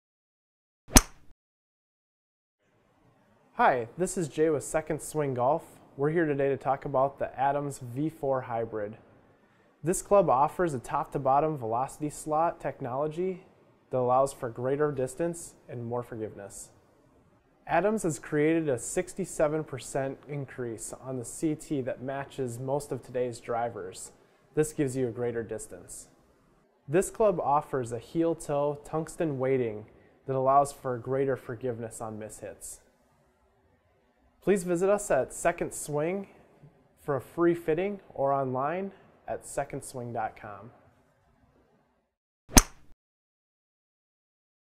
Adams Idea Tech V4 Hybrid Review
2nd Swing Golf club specialist Jay Sjovall reviews the 2012 Adams Idea Tech V4 Hybrid.